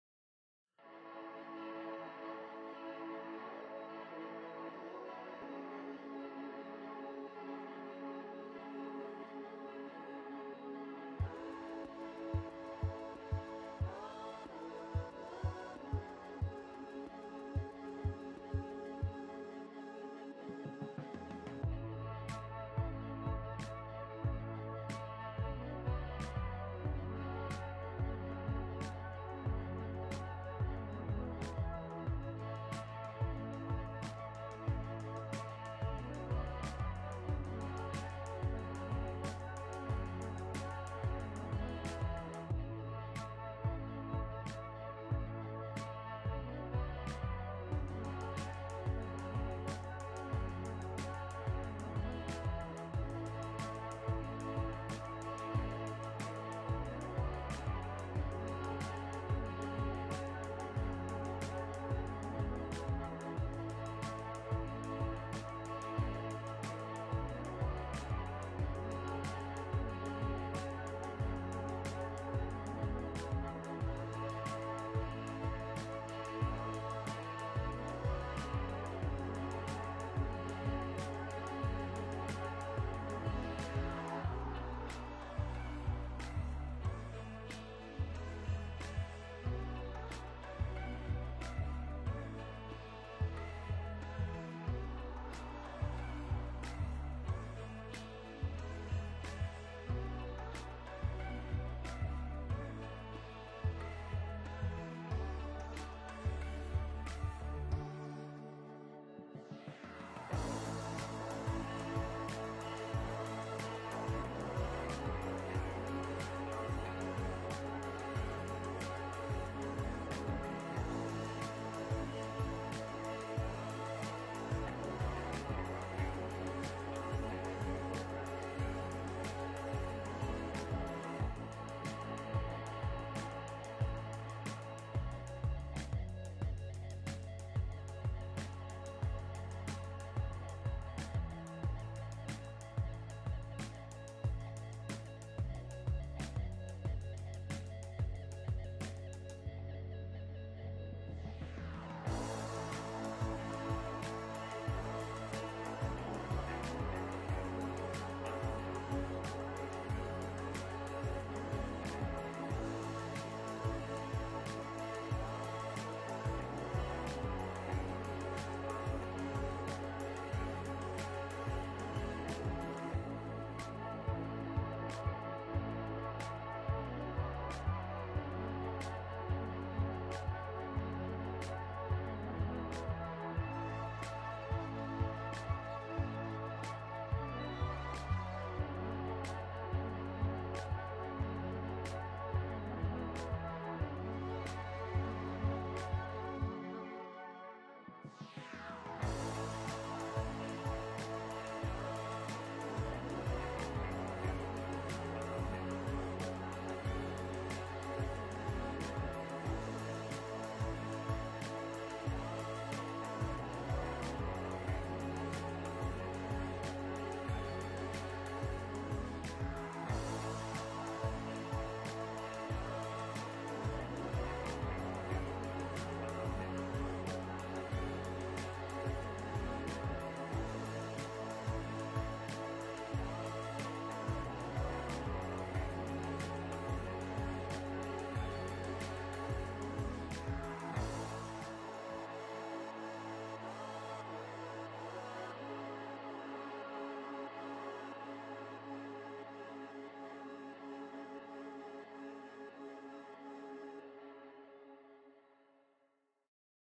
Watercolor fruit bowl (still life) - time-lapse
I did this painting this past week to show how how you can use watercolorblooms to your advantage in painting. Let the loose under layers happen :))